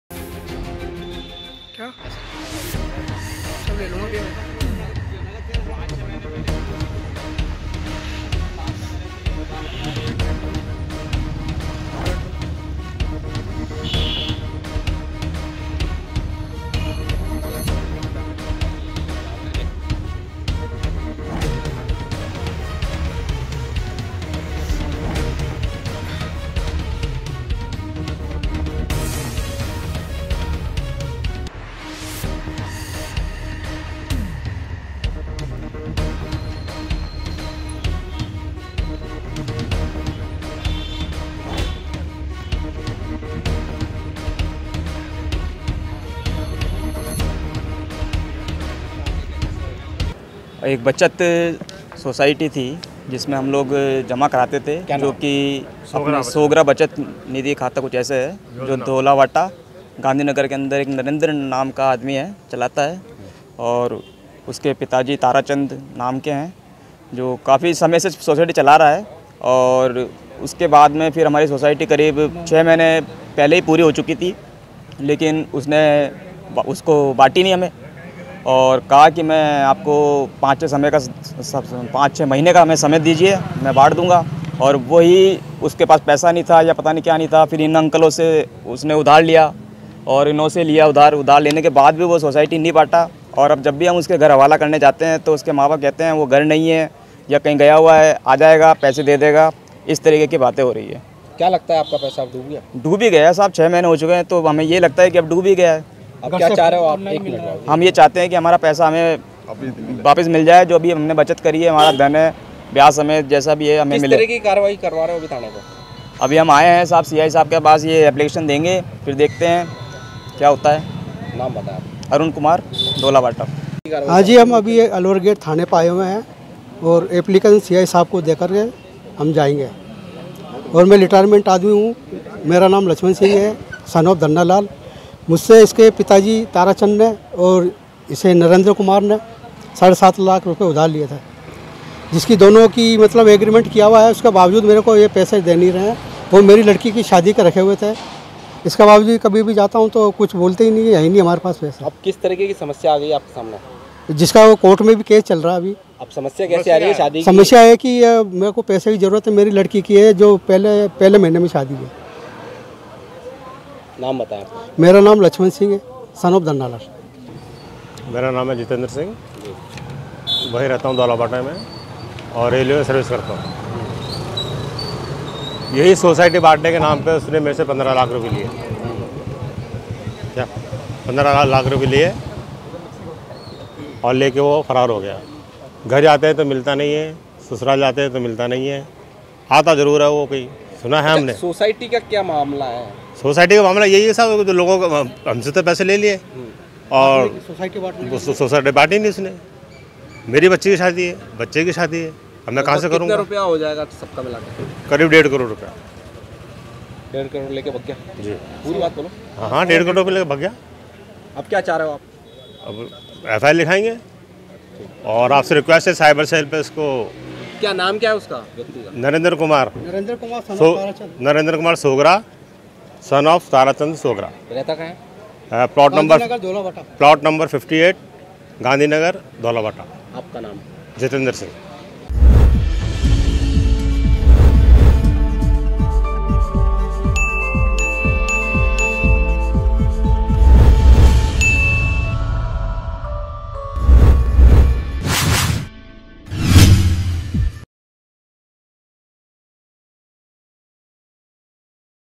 AJMER NEWS | धोलाभाटा इलाके से एक और बीसी संचालक हुआ फ़रार | MTTV INDIA
धोलाभाटा इलाके से एक और बीसी संचालक हुआ फ़रार
सैंकड़ों लोगों के लाखों रुपए लेकर हुआ फरार
पीड़ित लोगों ने अलवर गेट थाने में करवाया मामला दर्ज
सोगरा बचत योजना के संचालक नरेंद्र कुमार के खिलाफ मामला दर्ज
पीड़ितों के अनुसार आरोपी नरेंद्र करता है हवाले का काम